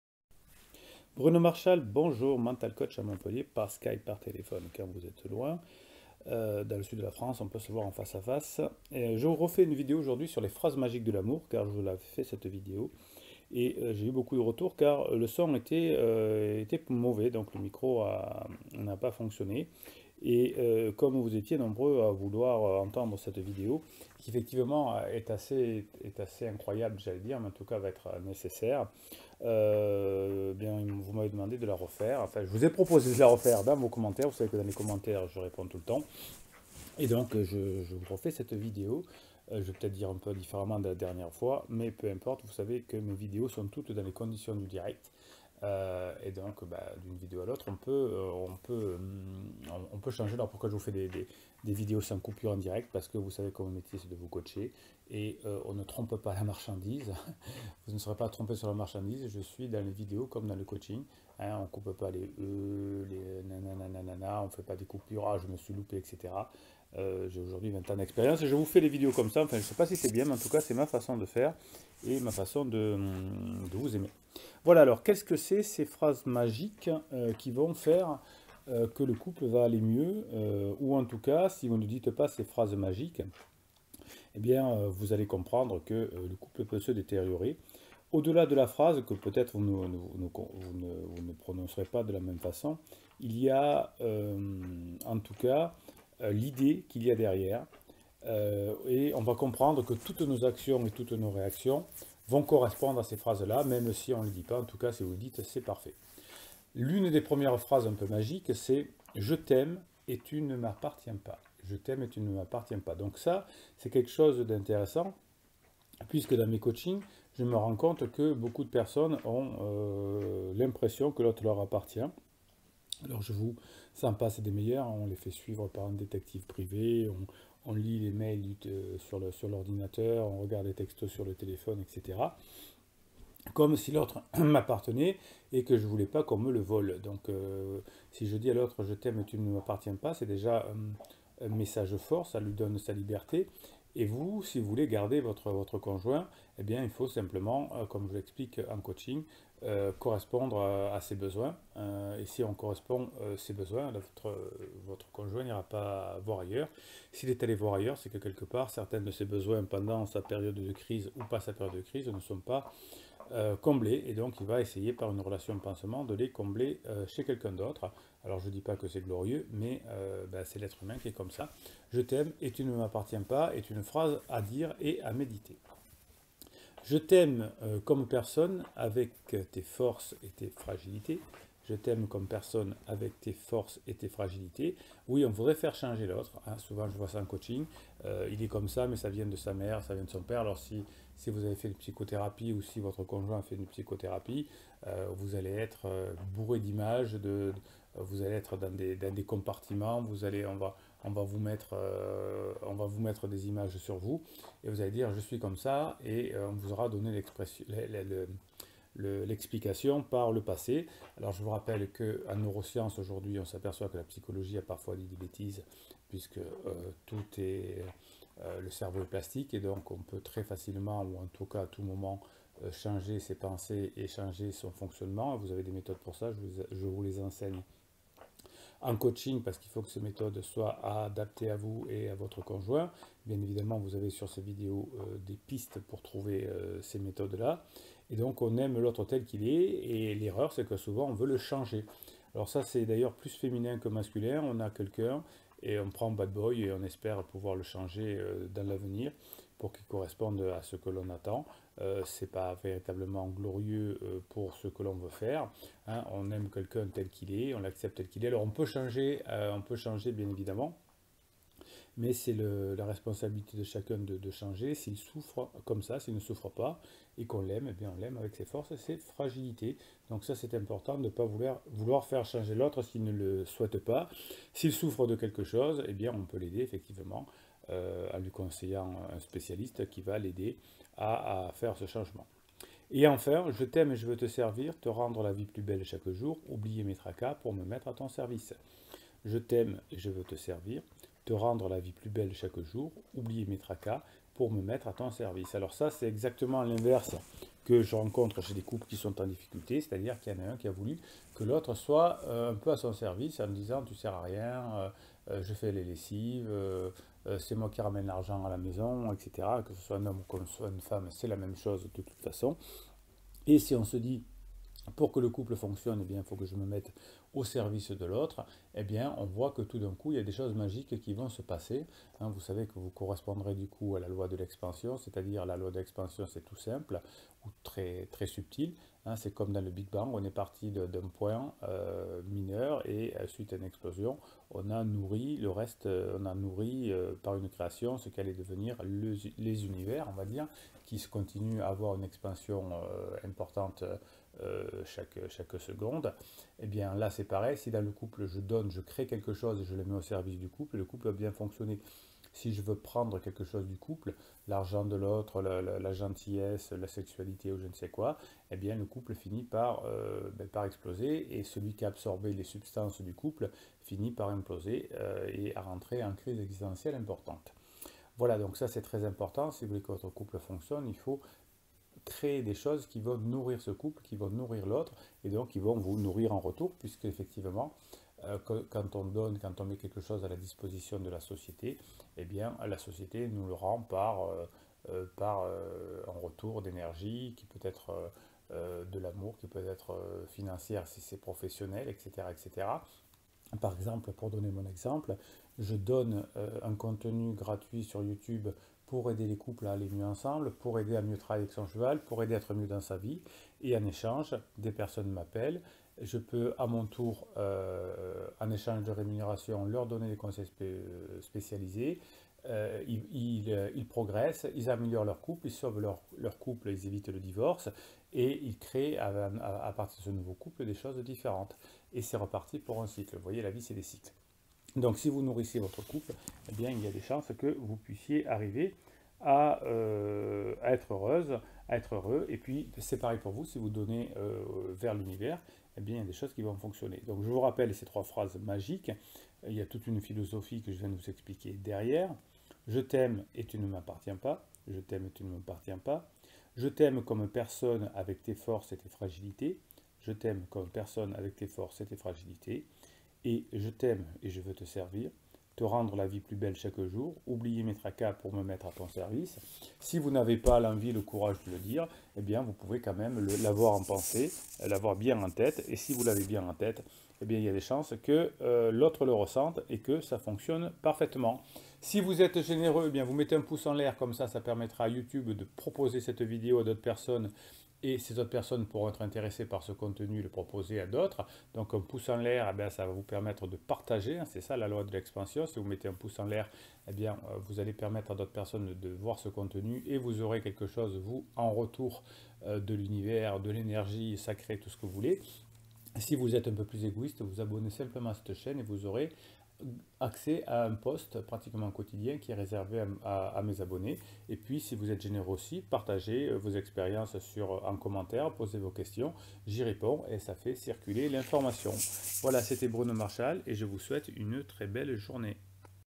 3 phrases magiques pour le couple
3 phrases magiques pour le couple

Vous avez envie de vous offrir un MP3 sur mesure de neuro hypnose? tout est expliqué ici

Suite à une erreur sur le son de la vidéo précédente je vous refait le contenu de la vidéo... si ca peut vous aider
« Du premier regard jusqu’aux noces d’or, les mots construisent nos couples… ou les détruisent !
Il existe des phrases qui permettent de mieux comprendre l’autre et dialoguer autour de ce qui peut l'aider.

Être en couple est une chance formidable ! Tant de célibataires cherchent l’amour et se désespèrent de ne jamais le trouver. Tant de couples pourtant gaspillent ce joyau inestimable. Que faire quand on ne maîtrise plus rien et que l’amour fuit notre couple comme l’eau dans un sceau percé ?

Et si il existait  des phrases magiques qui correspondent à la nature humaine?